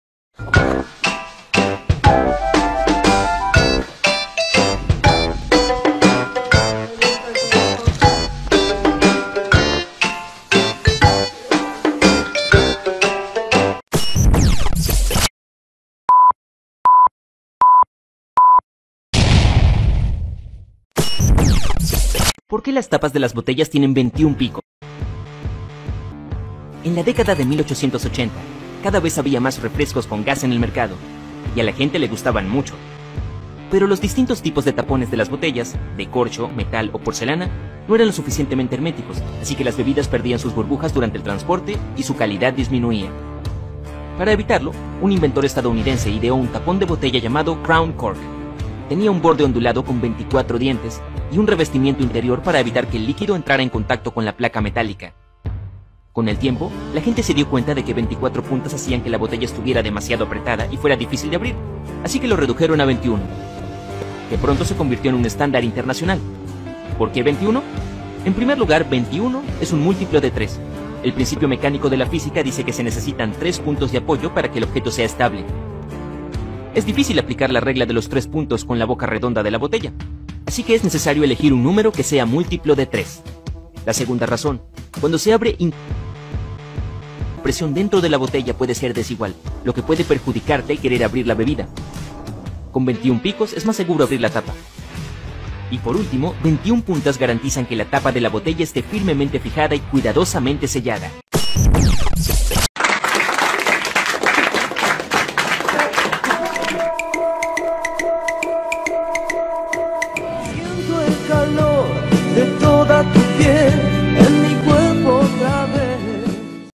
¿SABIAS POR QUE LAS TAPAS DE LAS BEBIDAS TIENEN 21 PUNTAS?
un entretenido relato de la idea de como surgio poner 21 puntas a las tapitas de algunas bebidas gaseosas y otras bebidas.